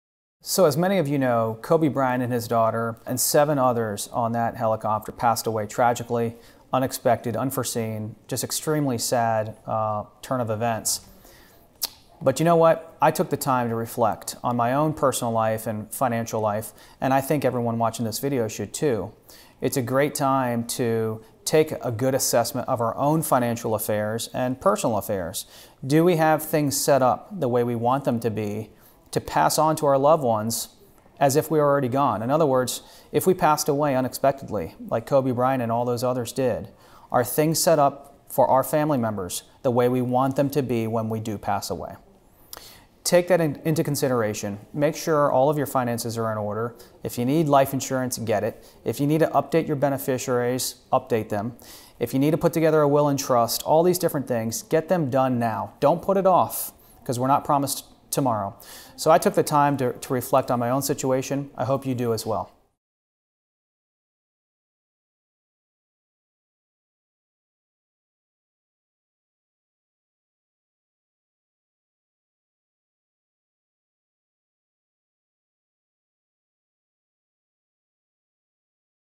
Are Your Finances In Order?
When tragedies strike, we need to remember that it could have been us. If your will and trust need to be updated, please give us a call. Always be prepared for the worst, regardless of what tomorrow holds.

So as many of you know, Kobe Bryant and his daughter and seven others on that helicopter passed away tragically. An unexpected, unforeseen, just extremely sad turn of events. But you know, what I took the time to reflect on my own personal life and financial life, and I think everyone watching this video should too.

It’s a great time to take a good assessment of our own financial affairs and personal affairs.

Do we have things set up the way we want them to be to pass on to our loved ones as if we are already gone? In other words, if we passed away unexpectedly like Kobe Bryant and all those others did, are things set up for our family members the way we want them to be when we do pass away?Take that into consideration.

Make sure all of your finances are in order. If you need life insurance, get it. If you need to update your beneficiaries, update them. If you need to put together a will and trust, all these different things, get them done now. Don’t put it off, because we’re not promised tomorrow.

So I took the time to reflect on my own situation I hope you do as well.